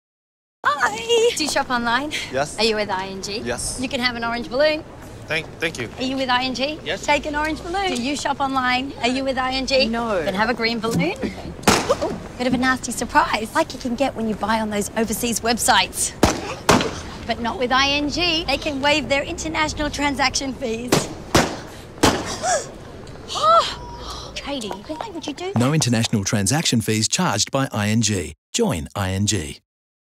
ING - Balloons
This fun ING commercial with Isla Fisher filmed with Hollywood Location at our exclusively represented properties.